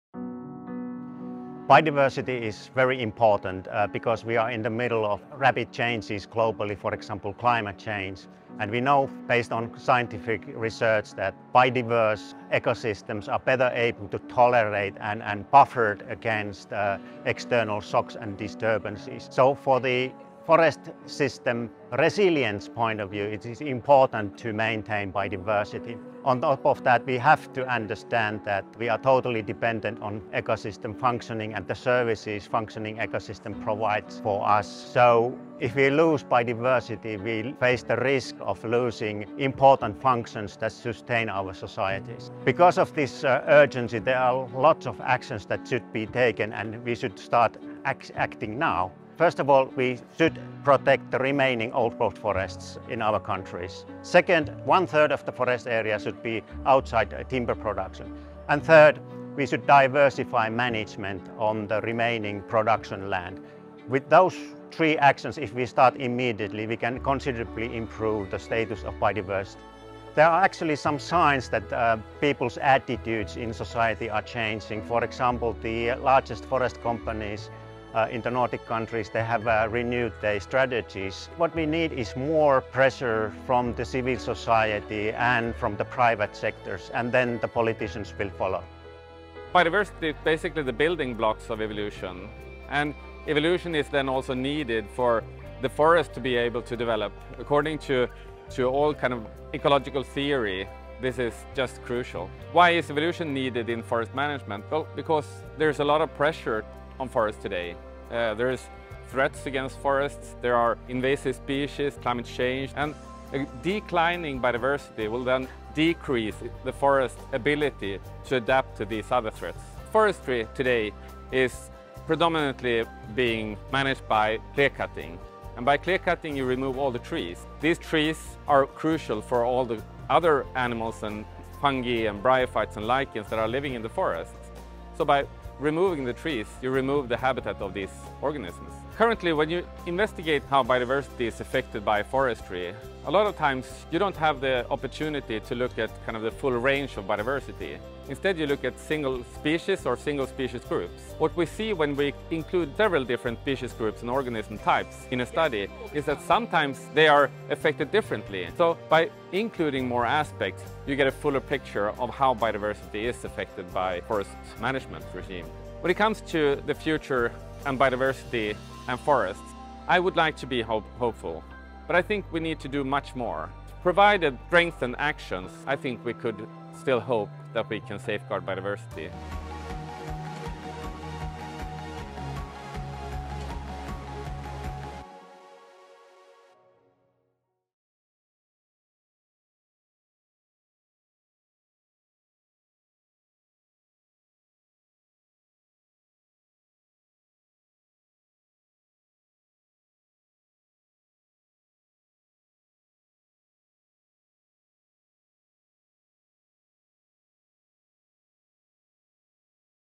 The Science of Safeguarding Biodiversity in Production Forestry
At the IUFRO World Congress we take a scientific look at biodiversity and hear from experts on how to build a forestry system that truly protects it. As biodiversity is increasingly under threat, is production forestry really well enough equipped to protect biodiversity? Mikko Mönkkönen (University of Jyväskylä) and Jörgen Sjögren (Swedish University of Agricultural Sciences) answer.